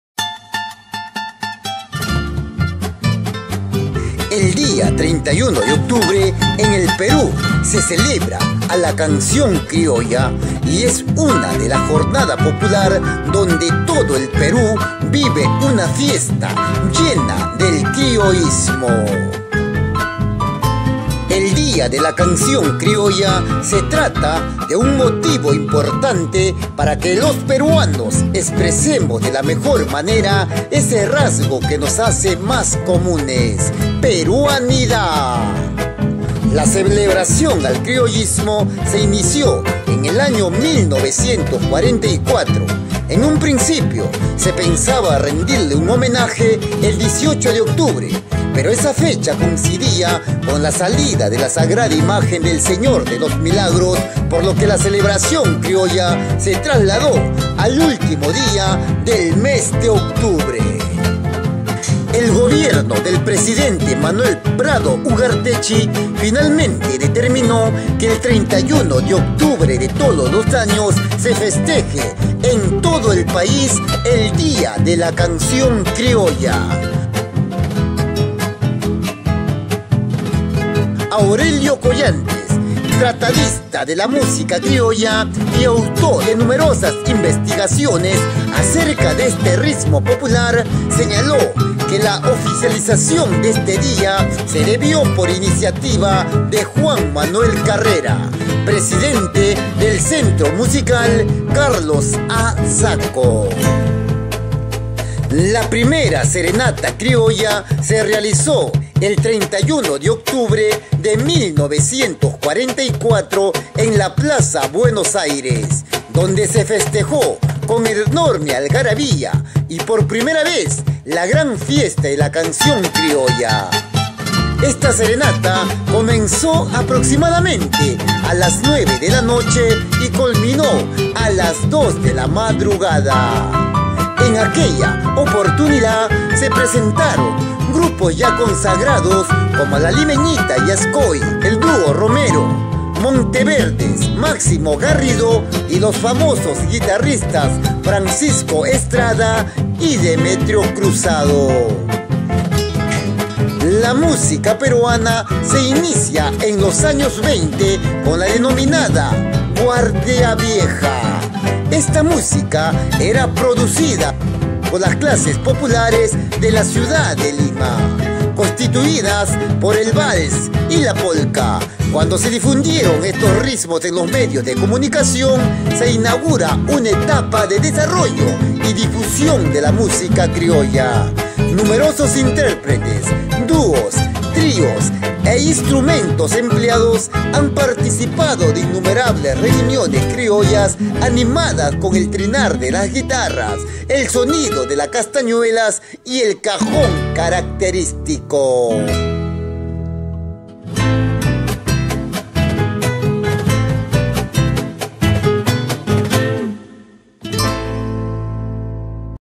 MV.FESTEJA DIA DE LA CANCION CRIOLLA!!!!!!!!!!!!!
31 DE OCTUBRE DIA DE LA CANCION CRIOLLA EN TODO EL PERU!